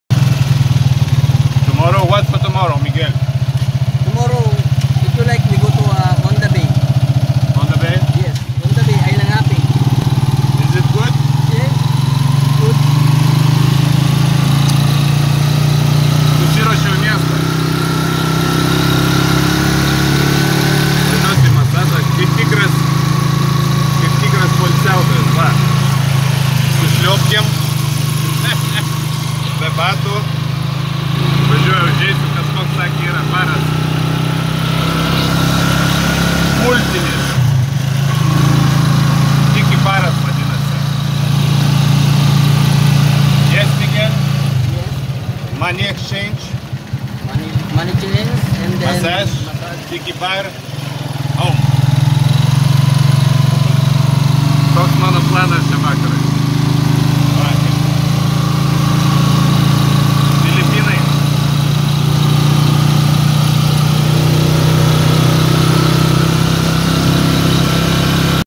Gyvenk įdomų gyvenimą. 53dalis
Filipinai. Puerto Princessa. "Atidirbau" visą dieną ekskursijose, nusikaliau- varau atsigaivinti :)